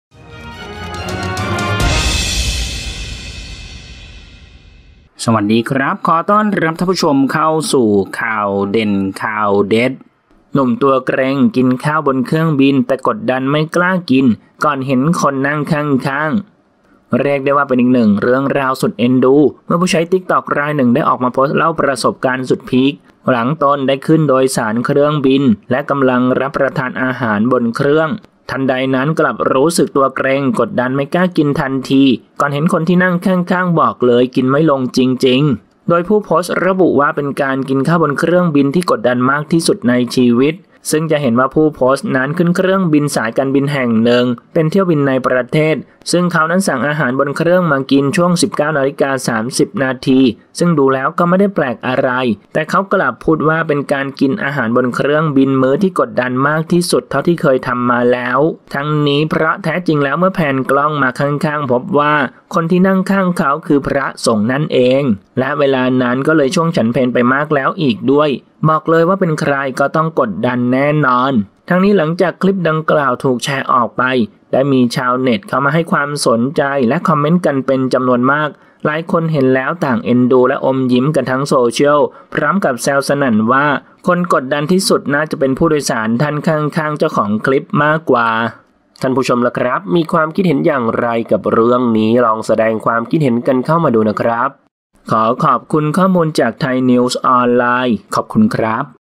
ชาวเน็ตแห่เอ็นดู หนุ่มกินข้าวบนเครื่องบิน แต่กดดันไม่กล้ากิน ก่อนเห็นคนนั่งข้าง กินไม่ลงจริงๆ
00:00 - Intro
00:10 - News
01:00 - Minute